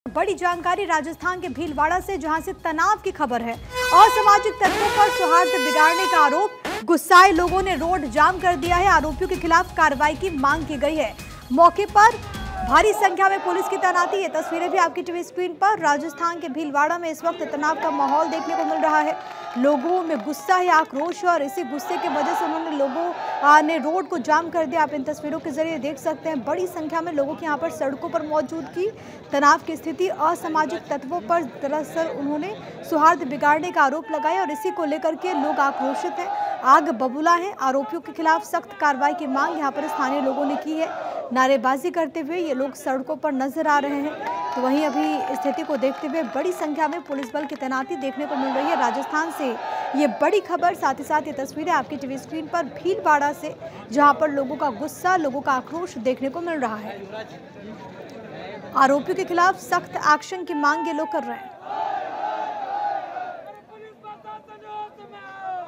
Breaking News : Rajasthan के Bhilwara में तनाव, गुस्साए लोगों ने रोड किया जाम
Anchor Name :Priya Sinha| Show Name :News | Time : 06: 00 PM | Date : 25/08/2024 | Published By:Yogesh Mishra